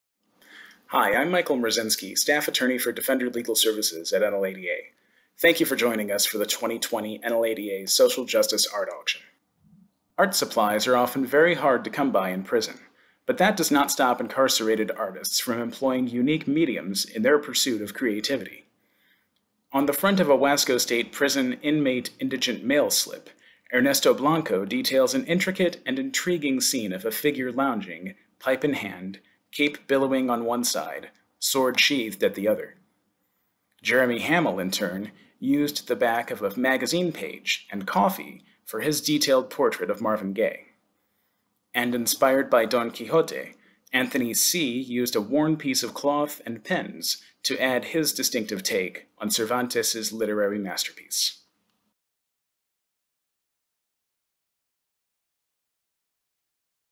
NLADA Social Justice Art Auction Video - Booth 6
NLADA staff social justice art curators highlight some of their favorite pieces and themes from our 2020 social justice art auction. Open through 9/17 at 1 a.m. ET.